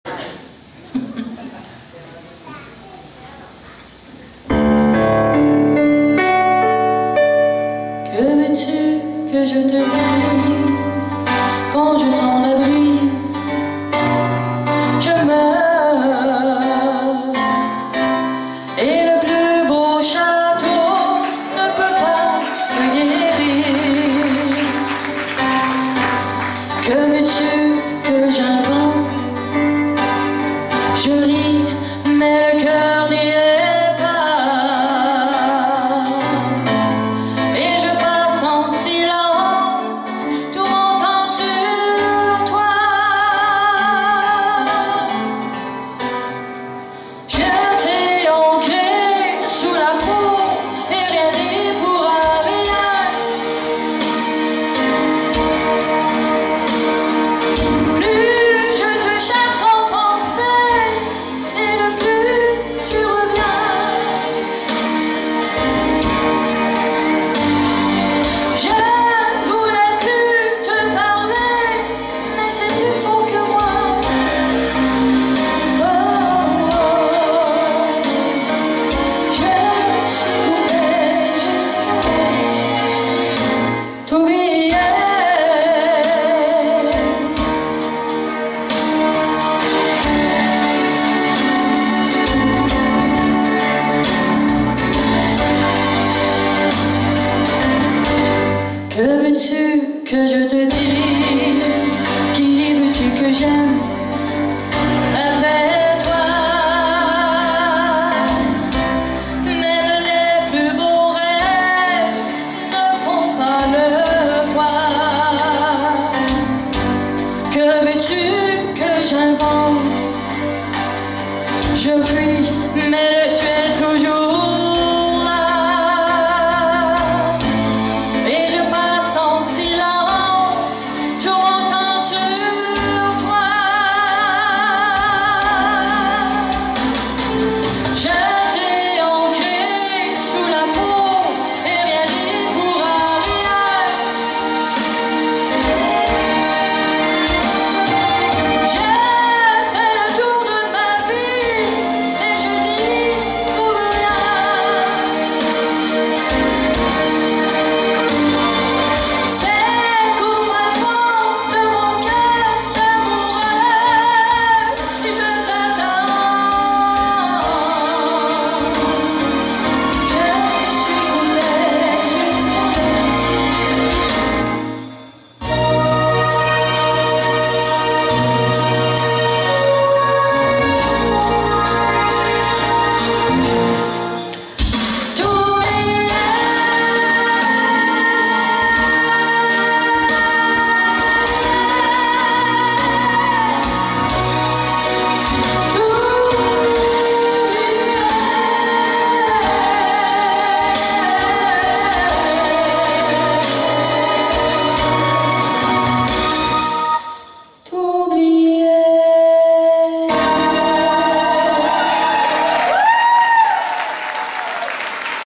Dominique Daoust 1.MPG
Dominique Daoust chante au spectacle de l'école de danse Nathalie Allard le 23 juin 2012. Deuxième prestation